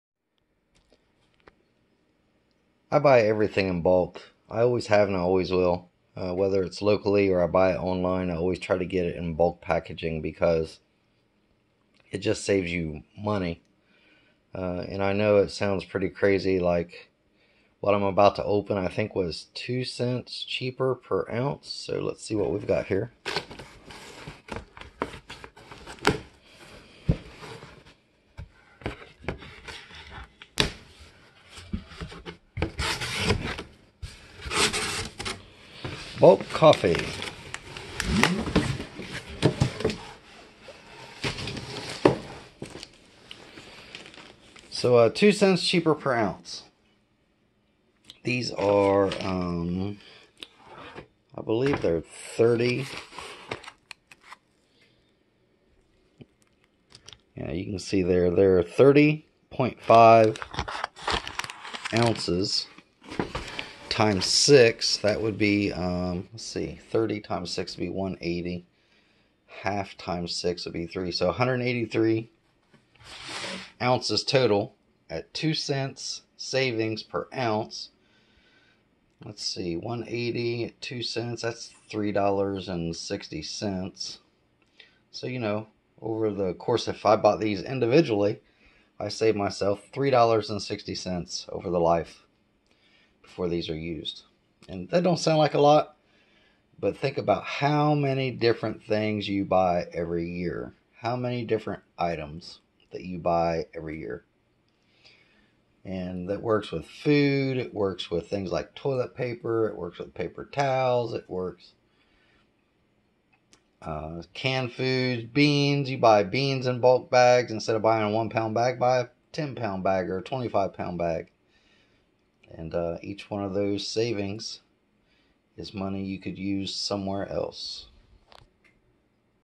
Buying in bulk on the homestead
I had planned to do a series this year called "my quest for self sufficiency and debt free" and one of the topics for that is squeezing every last drop of money out of things by buying in bulk. This is the first item I have had to purchase during the Corona virus event. I haven't been to a grocery store to actually buy things for the home since January and march is almost over already.  Some people would say you are hoarding but no, I've always bought things in bulk for years. I do it with household items, food, grain and feeds for livestock, etc. All those pennies add up over time.